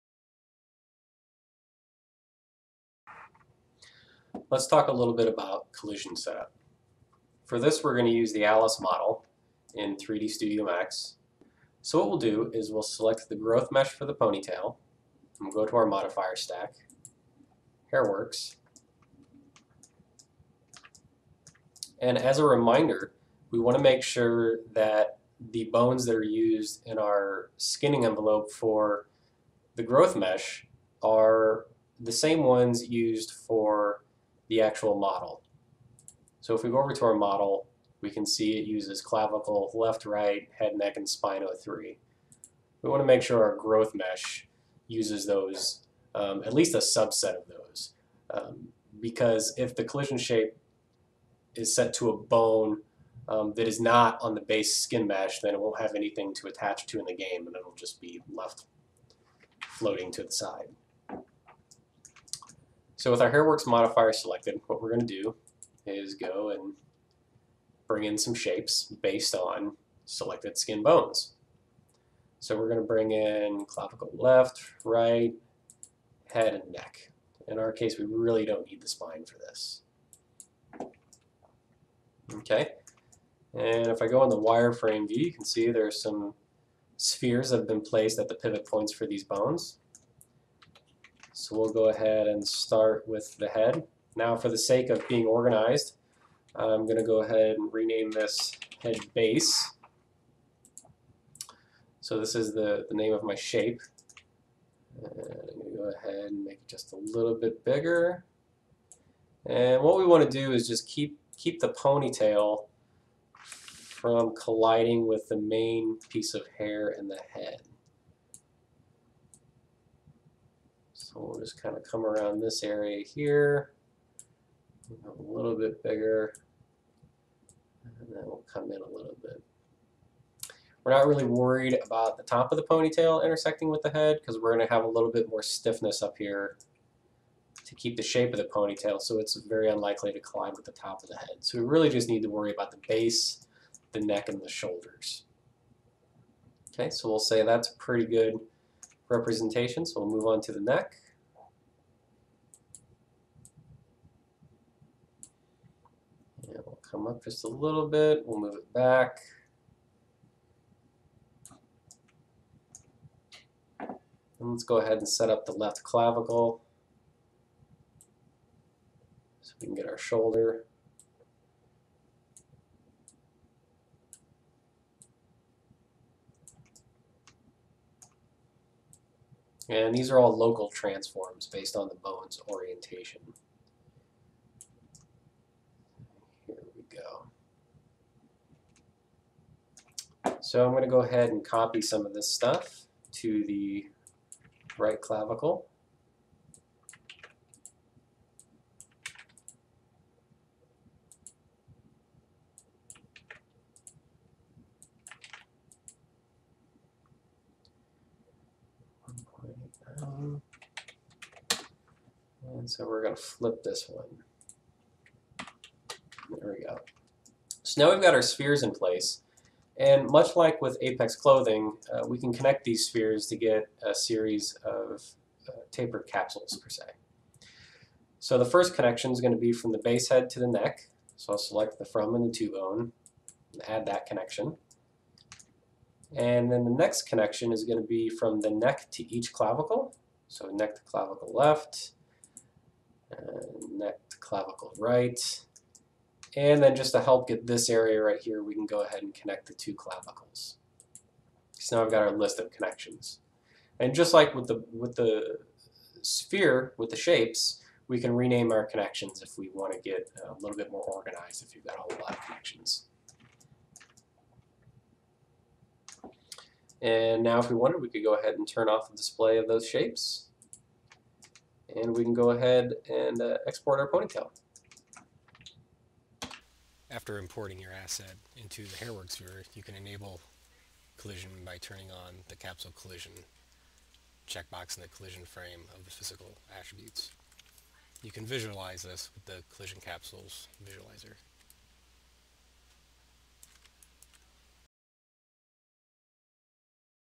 HairWorks r1.1.1 - Collision
This tutorial explain show to set up collision shapes and use them on your asset in 3dsMax and the HairWorks Viewer.